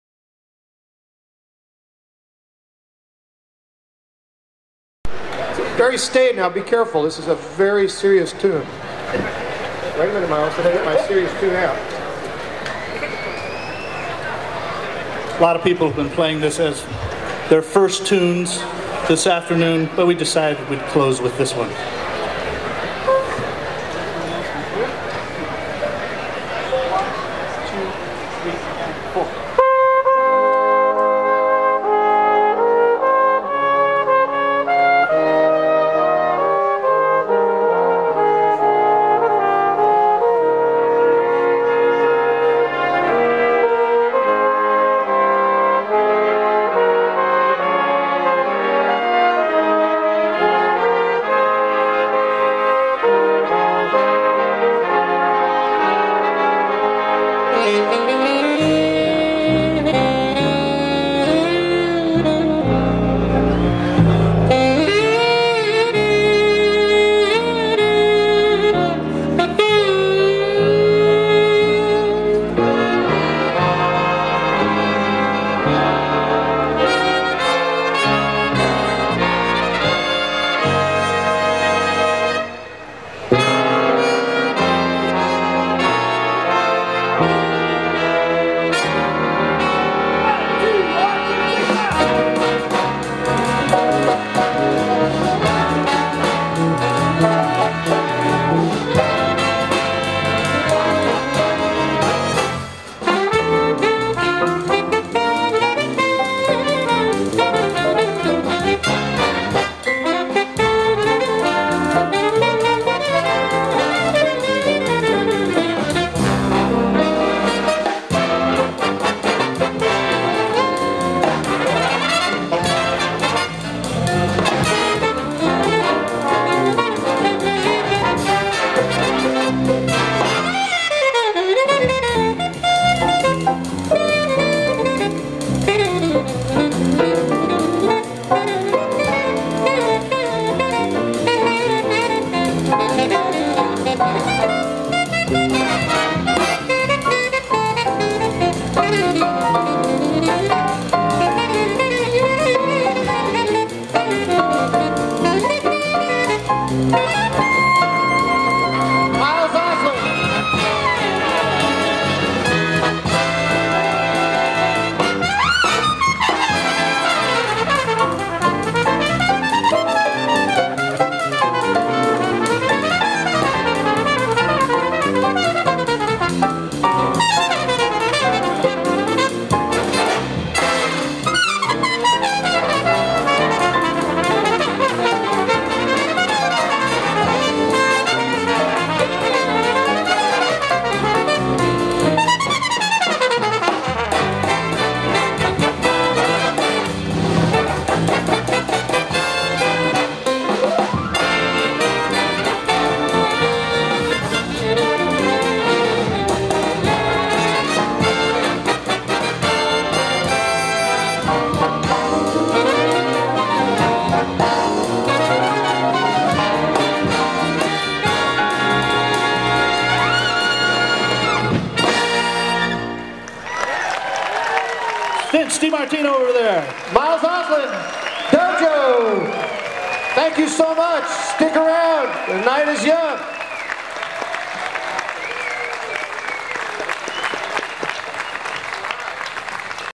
DOJO at the 2010 GABBF
Dimartino-Osland (DOJO) Jazz Orchestra at the 2010 Great American Brass Band Festival (GABBF)